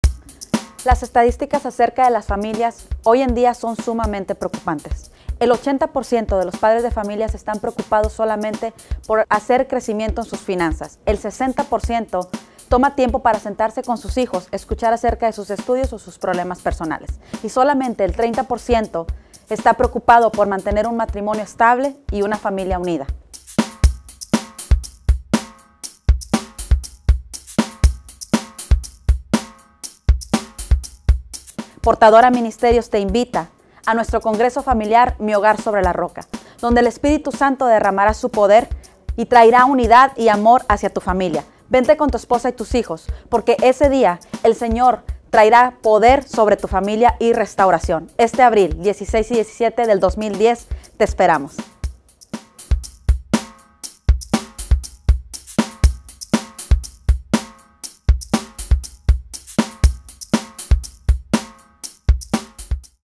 congreso familiar tu hogar sobre la roca
cdlf productions
Otto rene juarez

comercial del congreso familia tu hogar sobre la roca...
en catedral de la familia, houston texas